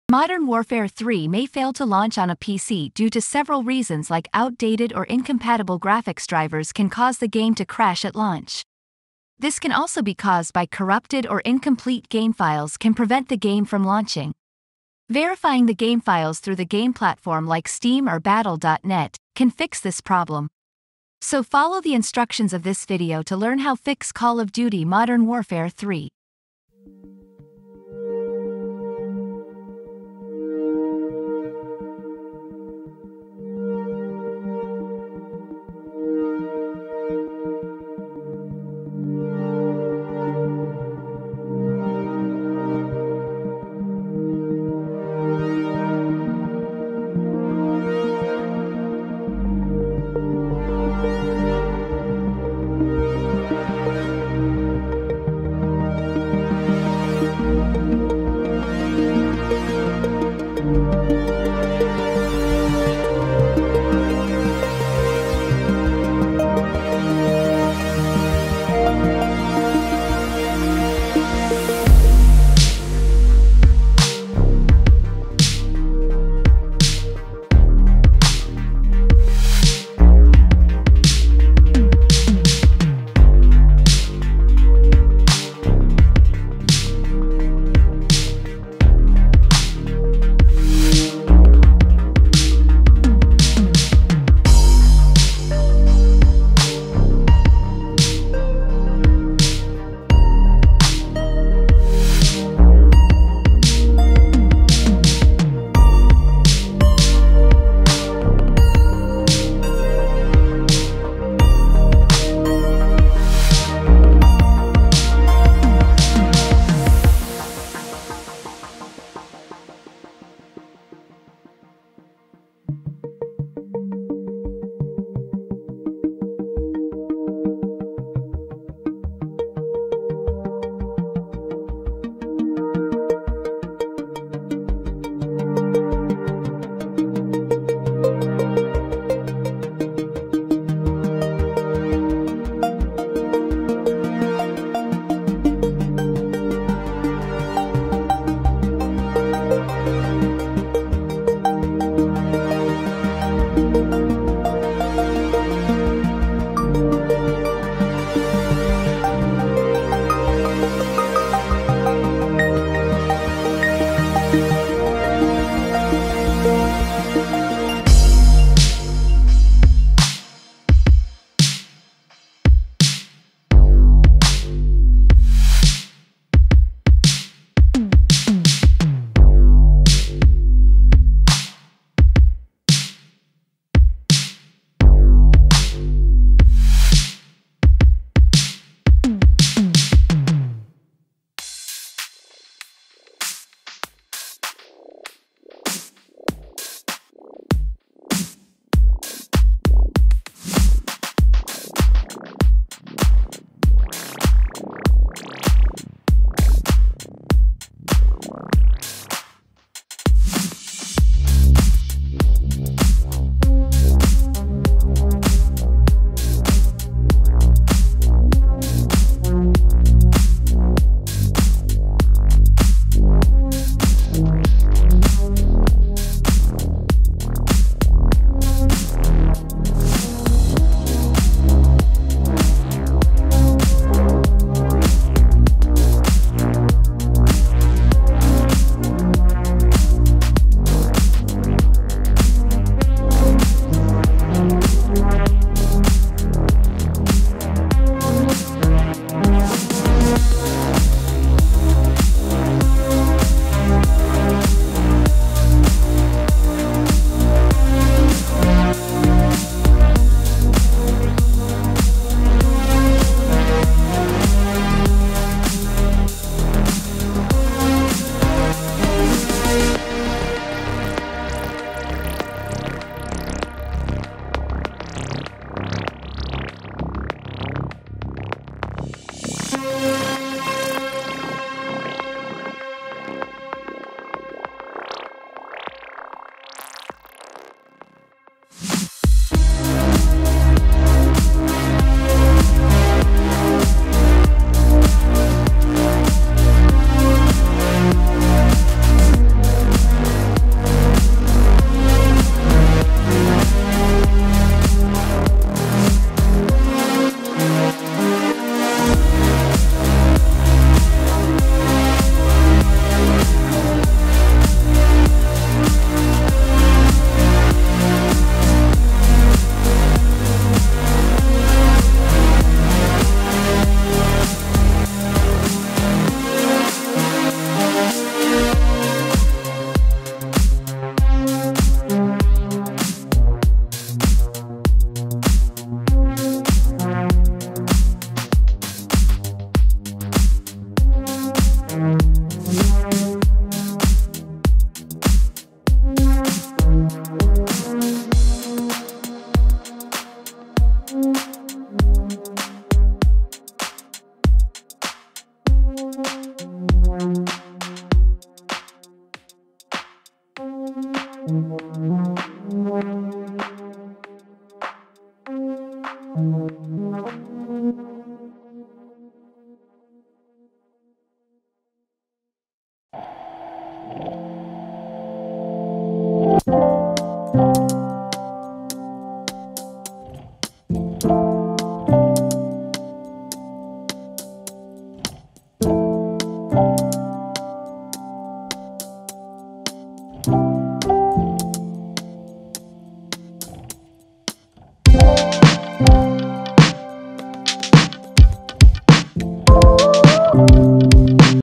Fix Call of Duty MW3 Not Launching / Not Working on Windows 11/10 PC
MW III may fail to launch on a PC due to several reasons like Outdated or incompatible graphics drivers can cause the game to crash at launch. this can also be caused by Corrupted or incomplete game files can prevent the game from launching. Verifying the game files through the game platform like Steam or Battle.net, can fix this problem. so follow the instructions of this video to learn how fix Call of Duty Modern Warfare 3 .

0:00 - Intro/Explanation
0:25 - Set Default GPU
1:06 - Update GPU Drivers
4:25 - Run MW3 in compatibility mode